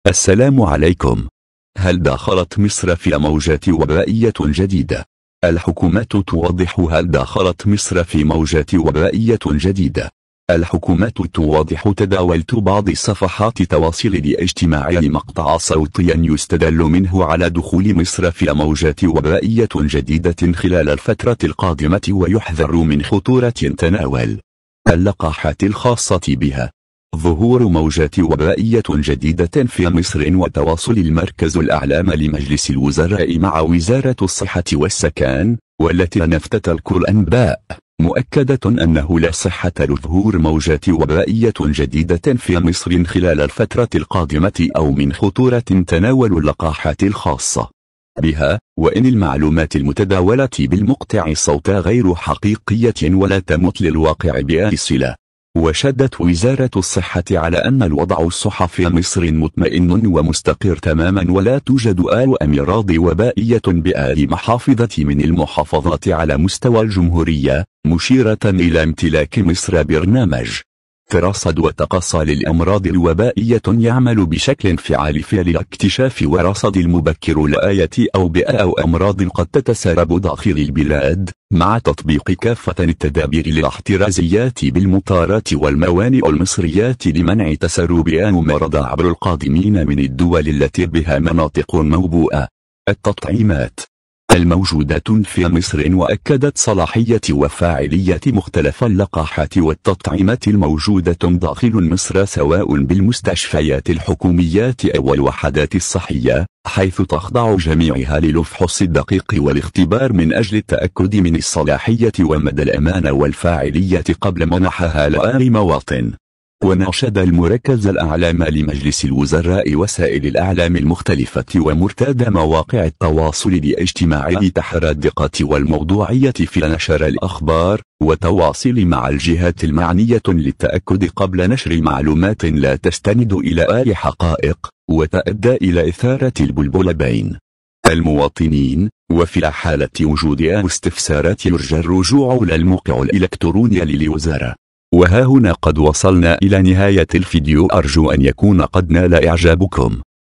هَلْ دَخَلَتْ مِصْرَ فِى مَوْجَاتِ وَبَائِيَّةٌ جَدِيدَةٍ؟..
هَلْ دَخَلَتْ مِصْرَ فِى مَوْجَاتِ وَبَائِيَّةٌ جَدِيدَةٍ؟.. «الْحُكُومَةُ» تُوضِحُ
هَلْ دَخَلَتْ مِصْرَ فِي مَوْجَاتِ وَبَائِيَّةٌ جَدِيدَةٍ؟.. «الْحُكُومَةُ» تُوضِحُ

تَدَاوَلَتْ بَعْضِ صَفَحَاتِ الْتَوَاصِلِ الإِجْتِمَاعِىِّ مَقْطَعَا صوتيًا يُسْتَدَلُّ مِنْهُ عَلَى دُخُولِ مِصْرَ فِى مَوْجَاتِ وَبَائِيَّةٌ جَدِيدَةٍ خِلَالَ الْفَتْرَةِ الْقَادِمَةِ وَيُحْذَرُ مِنْ خُطُورَةٍ تَنَاوَلُ اللَّقَاحَاتِ الْخَاصَّةِ بِهَا.
ظُهُورُ مَوْجَاتِ وَبَائِيَّةٌ جَدِيدَةٍ فِى مِصْرٍ

وَتَوَاصُلِ الْمَرْكَزُ الْأَعْلَامَى لِمَجْلِسِ الْوُزَرَاءِ مَعَ وِزَارَةُ الصِحَةِ وَالسَّكَانِ، وَالَّتِى نَفْتَ تَلْكُ الْأَنْبَاءِ، مُؤَكَّدَةٌ أَنَّهُ لَا صِحَّةَ لِظُهُورِ مَوْجَاتِ وَبَائِيَّةٌ جَدِيدَةٍ فِى مِصْرٍ خِلَالَ الْفَتْرَةِ الْقَادِمَةِ أَوْ مِنْ خُطُورَةٍ تَنَاوَلُ اللَّقَاحَاتِ الْخَاصَّةِ بِهَا، وَإِنْ الْمَعْلُومَاتِ الْمُتَدَاوَلَةِ بِالْمَقْطَعِ الصوتى غَيْرُ حَقِيقِيَّةٍ وَلَا تَمُتْ لِلْوَاقِعِ بِأَىِّ صِلَةٌ.

وَشَدَدْتُ وِزَارَةُ الصِّحَّةِ عَلَى أَنَّ الْوَضْعُ الصحى فِى مِصْرٍ مُطْمَئِنٌّ وَمُسْتَقِرُّ تَمَامًا وَلَا تُوجَدُ أَىُّ امِرَاضِ وَبَائِيَّةٌ بِأَىِّ مَحَافِظَةِ مِنْ الْمُحَافَظَاتِ عَلَى مُسْتَوَى الْجُمْهُورِيَّةِ، مُشِيرَةً إِلَى امْتِلَاكِ مِصْرَ بِرْنَامَجِ تَرَصَّدَ وَتَقَصَّى لِلْأَمْرَاضِ الْوَبَائِيَّةٌ يَعْمَلُ بِشَكْلٍ فِعَالِ فِى الِاكْتِشَافِ وَالرَّصَدِ الْمُبَكِّرُ لَايَةِ أوبئة أَوْ أَمْرَاضٍ قَدْ تَتَسَرَّبُ دَاخِلِ الْبِلادِ، مَعَ تَطْبِيقِ كَافَّةً التَّدَابِيرِ الاحْتِرَازِيَّةِ بِالْمَطَارَاتِ وَالْمَوَانِئُ الْمِصْرِيَّةِ لِمَنْعِ تَسَرُّبِ أَىُّ مَرَّضَ عَبْرَ الْقَادِمِينَ مِنْ الدُّوَلِ الَّتِى بِهَا مَنَاطِقٌ مَوْبُوءة.

التطعيمات الْمَوْجُودَةٌ فِى مِصْرٍ

وَأَكَّدَتْ صَلَاحِيَّةِ وَفَاعِلِيَّةِ مُخْتَلَفَ اللَّقَاحَاتِ والتطعيمات الْمَوْجُودَةٌ دَاخِلٌ مِصْرَ سَوَاءٌ بِالْمُسْتَشْفَيَاتِ الْحُكُومِيَّةِ اوَ الْوَحَدَاتِ الصِّحِّيَّةِ، حَيْثُ تَخْضَعُ جَمِيعِهَا لِلْفَحْصِ الدَّقِيقِ وَالِاخْتِبَارِ مِنْ أَجْلِ التَّأَكُّدِ مِنْ الصَّلَاحِيَّةِ وَمَدَى الْأَمَانَ وَالْفَاعِلِيَّةِ قَبْلَ مَنَحَهَا لأَىِّ مَوَاطِنَ.

وَنَاشَدَ الْمُرَكَّزَ الْأَعْلَامَى لِمَجْلِسِ الْوُزَرَاءِ وَسَائِلِ الْإِعْلَامِ الْمُخْتَلِفَةِ ومرتادى مَوَاقِعِ التَّوَاصُلِ الإِجْتِمَاعِىِّ تَحَرَّى الدِّقَّةِ وَالْمَوْضُوعِيَّةِ ‏فِى نَشَرَ الْأَخْبَارِ، وَالْتَوَاصِلِ مَعَ الْجِهَاتِ الْمَعْنِيَّةٌ لِلتَّأَكُّدِ قَبْلَ نَشْرِ ‏مَعْلُومَاتٍ لَا تَسْتَنِدُ إِلَى أَىِّ حَقَائِقِ، وَتُؤَدَّى إِلَى إِثَارَةِ الْبَلْبَلَةُ بَيْنَ الْمُوَاطِنِينَ، وَفِى حَالَةِ وُجُودِ أَىُّ اسْتِفْسَارَاتِ يُرْجَى الرُّجُوعُ لَلْمُوقِعُ الْإِلَكْترُونِى لِلْوَزَارَةَ